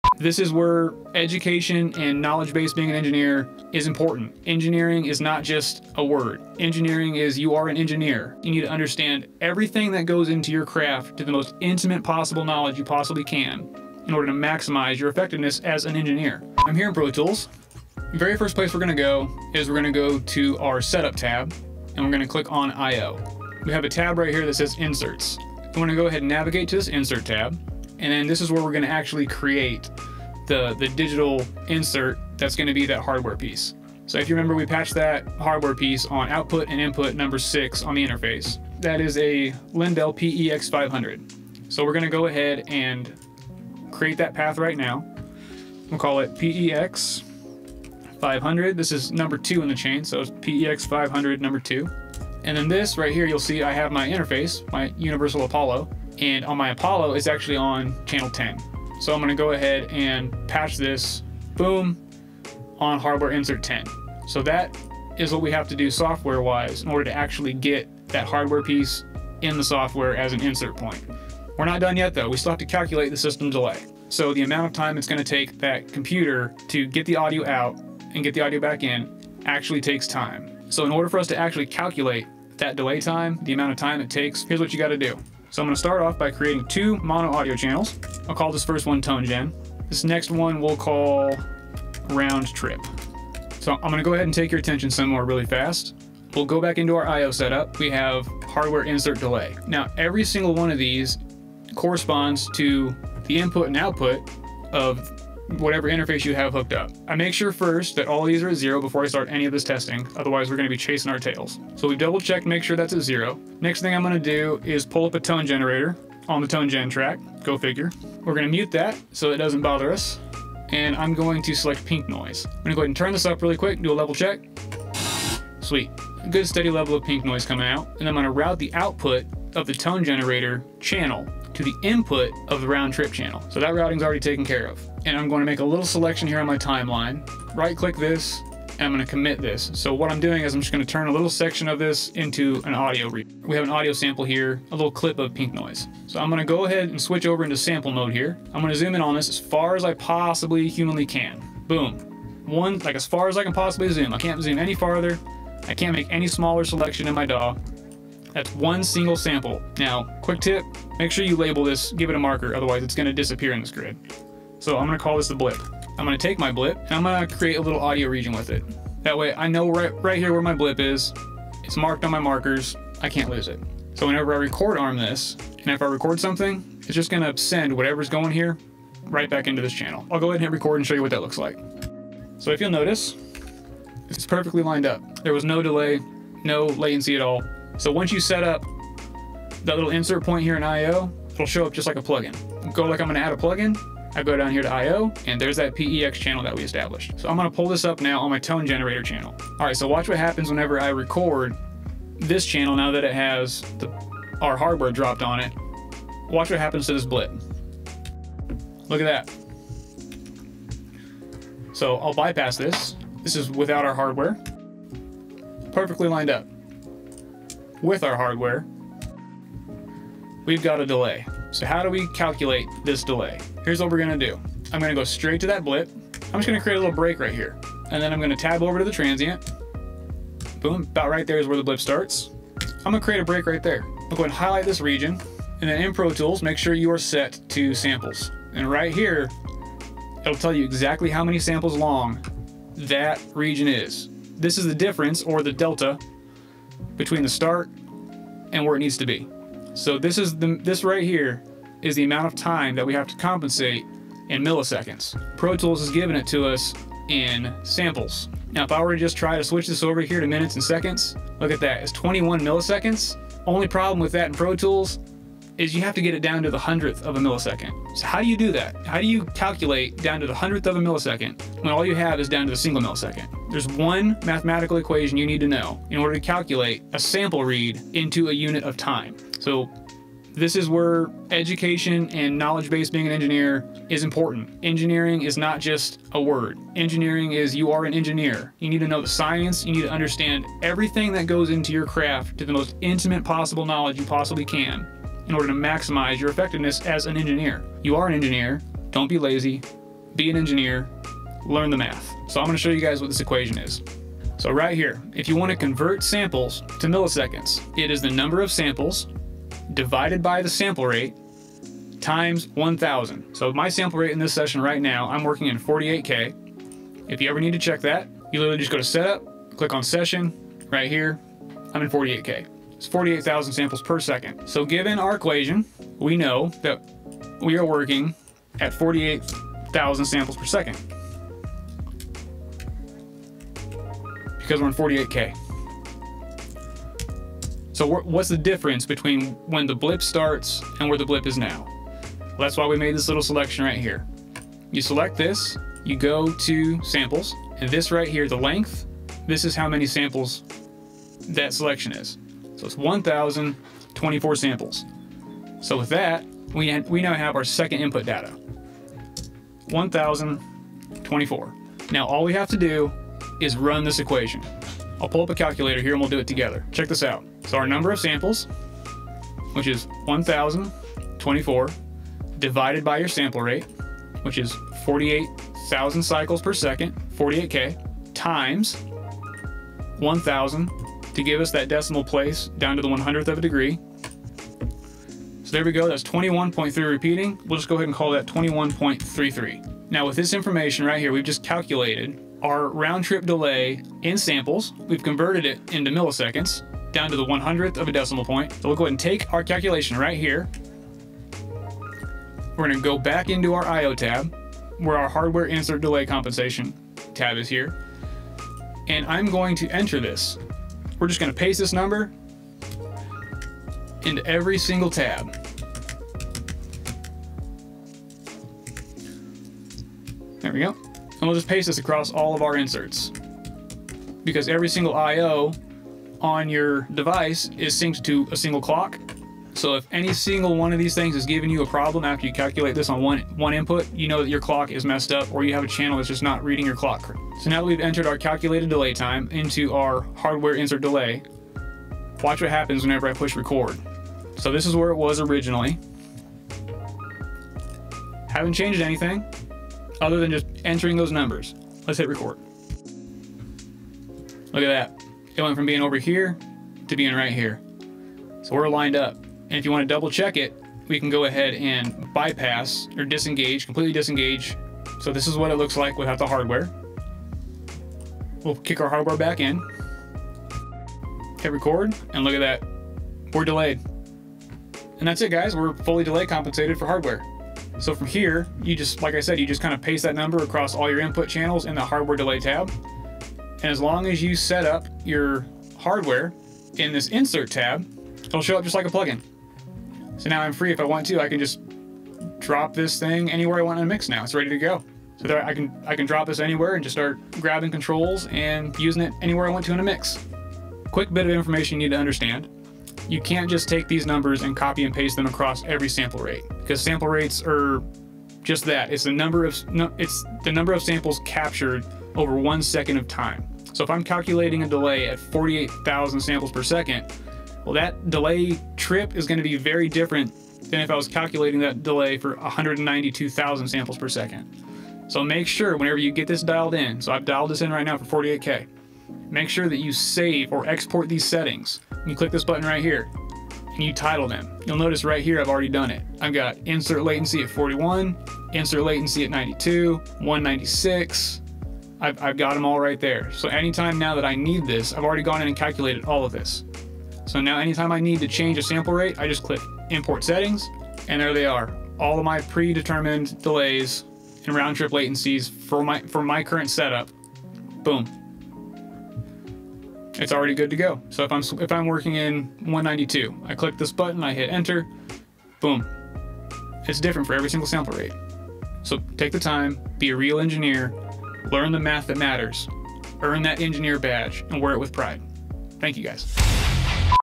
Setting Up Hardware: The DAW Side - Protools Inserts And Hardware Delay Compensation
Let’s Deep Dive: setting up ProTools to use hardware as inserts and calculating hardware insert delay compensation.